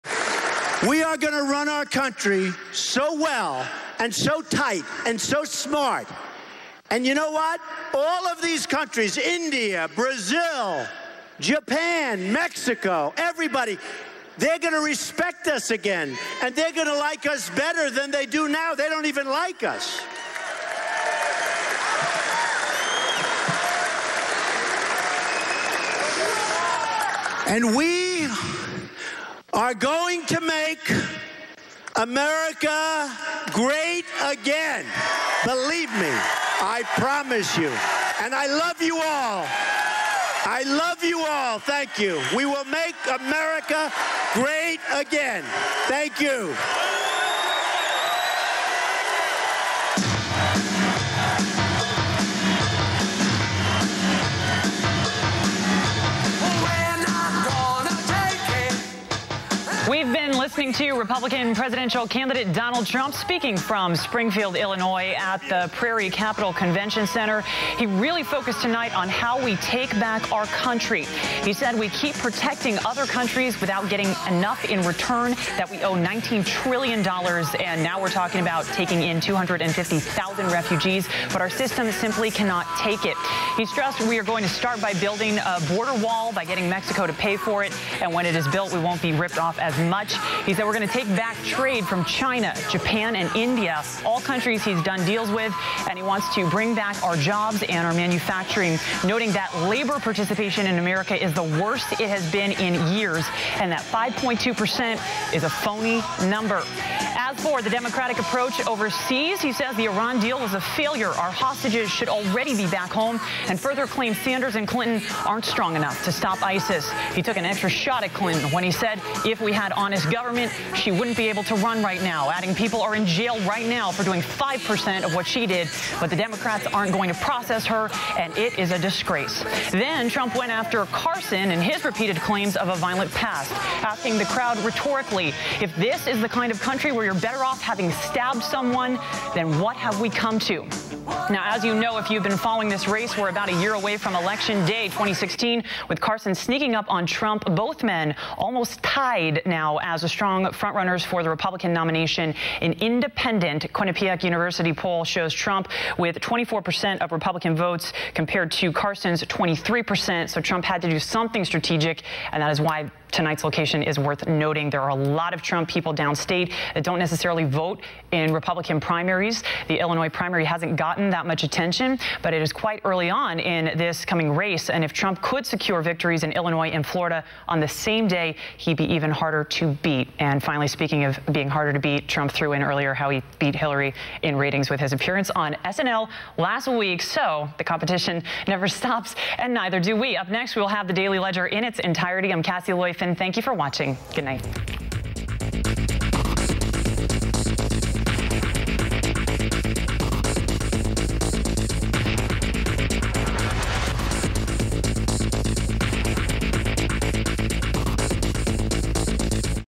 TRUMP speaking LIVE in Springfield, IL - CASSIE LEUFFEN summarizes
Monday, November 9th, 2015
Cassie Leuffen summarizes Trump's hour-long rally in Springfield IL.